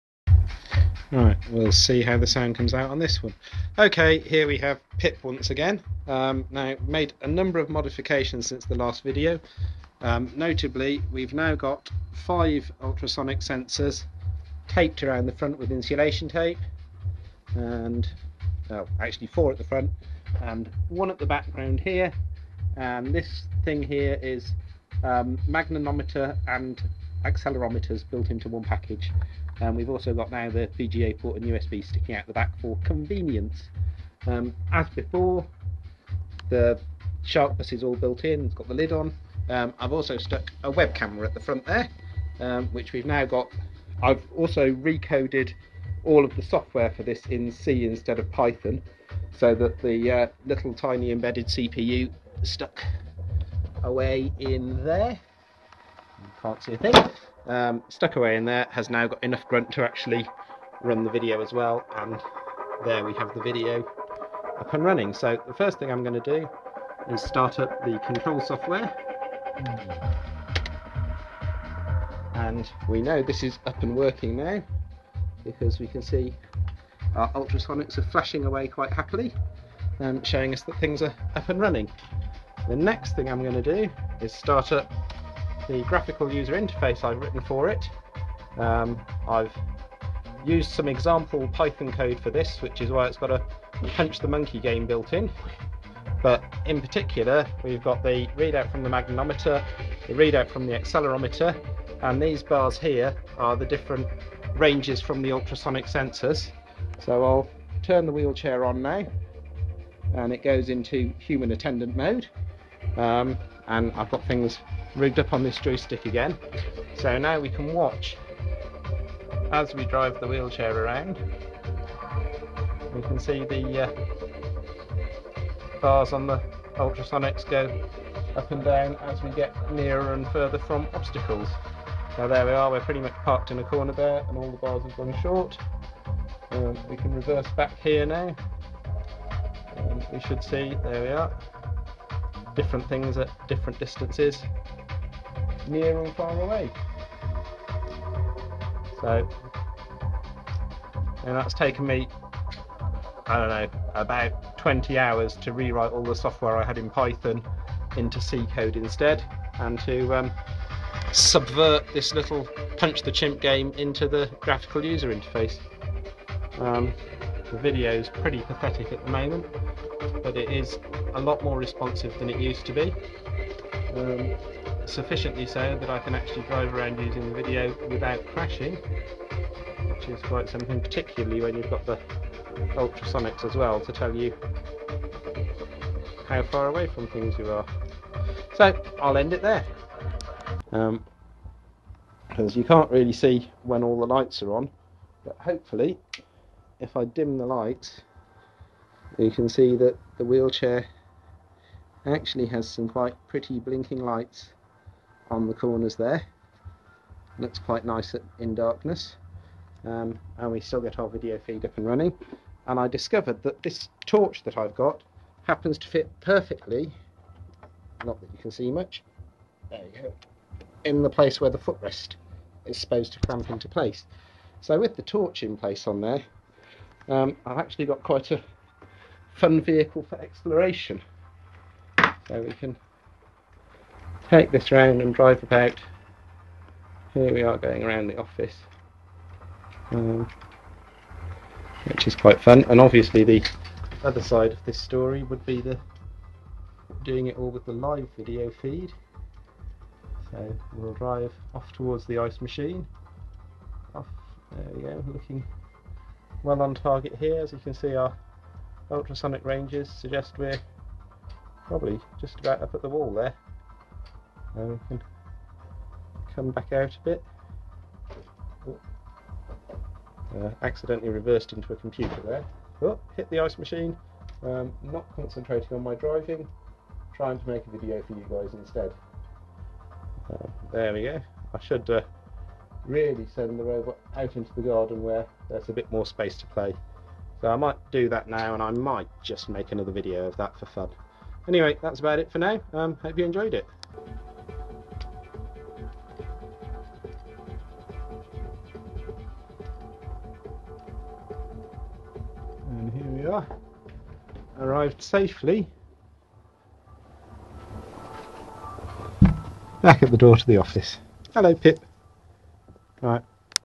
Hacking an Invacare Pronto Electric Wheelchair - Part 9 (newly discovered footage from 2010)
Newly discovered footage of Pip the robot wheelchair - These clips have sat on an old phone since October 2010 (four years ago) without ever being seen - I just rediscovered them - This is probably the best footage I got of Pip.

Technical Details:
Invacare Pronto Wheelchairs used an RS485 multidrop serial bus and a protocol called SharkBus for the communications from the joystick to the motor controller unit. I reverse engineered the SharkBus Protocol and mounted a small embedded linux PC inside the base, along with wifi, webcam, accelerometers and ultrasonic distance sensors to make a rather fun creation. never did get around to adding an apple collecting scoop though...